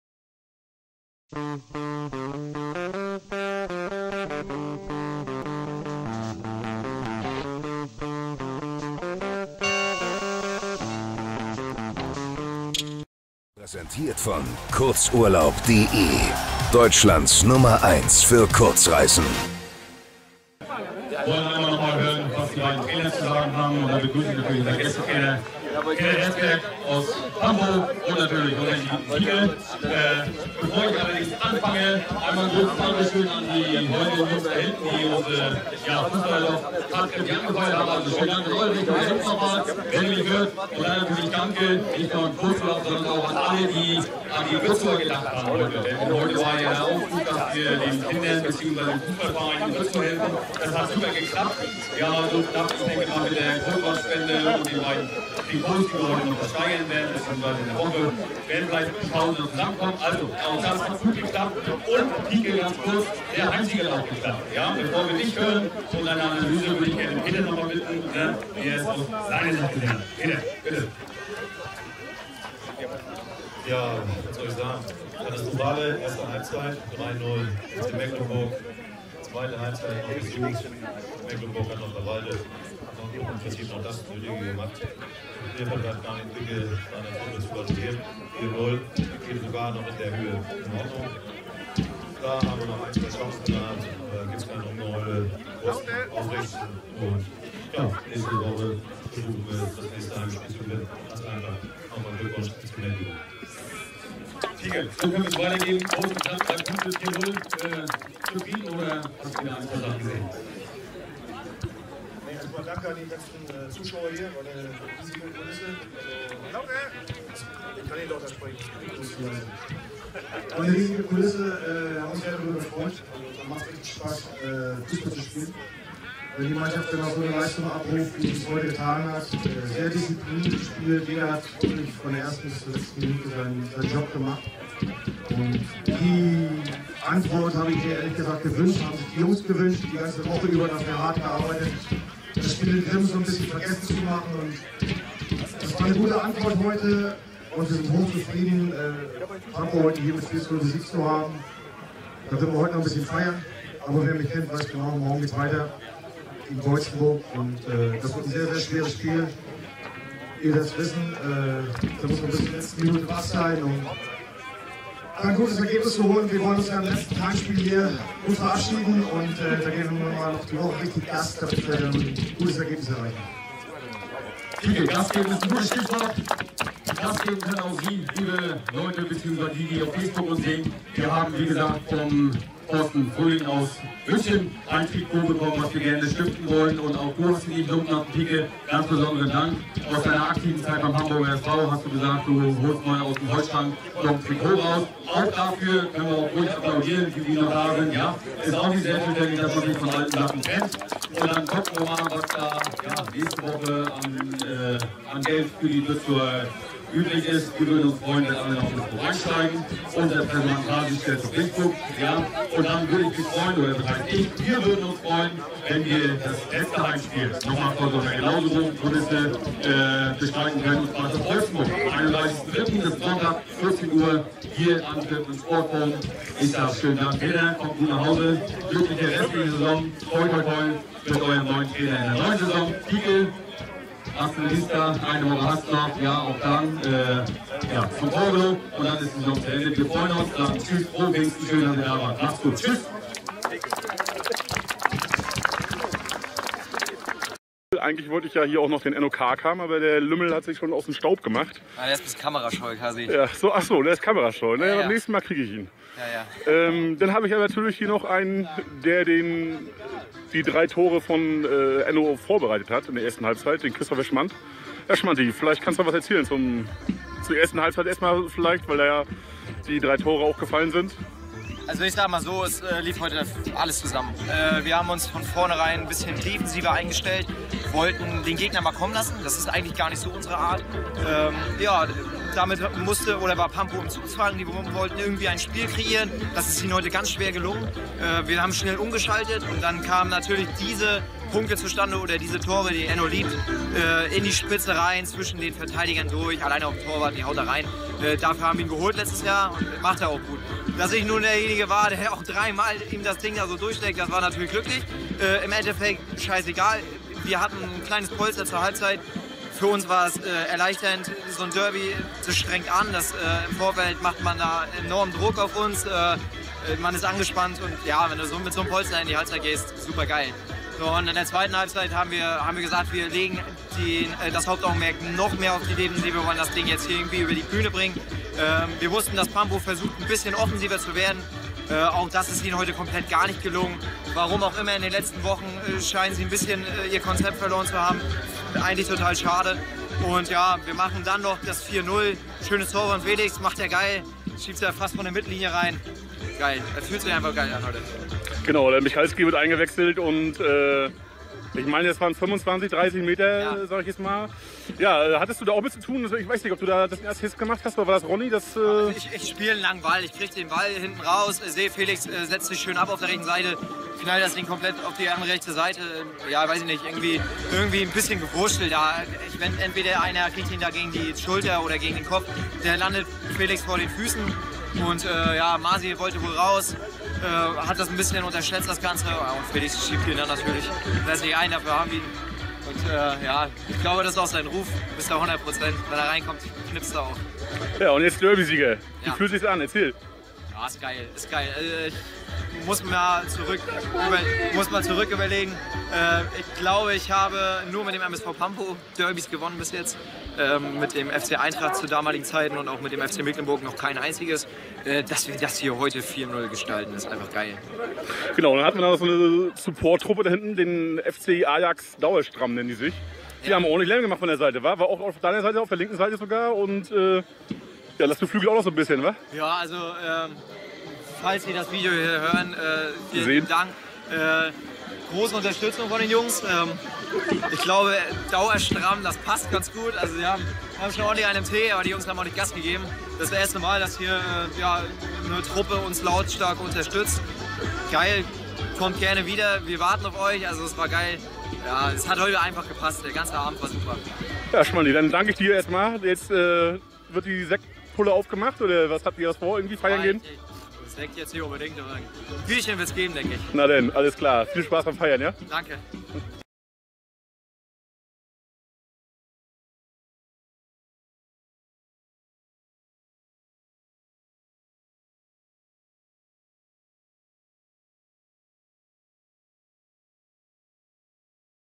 Stimmen zum Derby: FCM Schwerin - MSV Pampow
Verbandsliga MV 2014/15
28.Spieltag
FC Mecklenburg Schwerin - MSV Pampow
Endstand 4:0
Stimmen zum Spiel
Peter Herzberg
Martin Pieckenhagen
Christopher Schmandt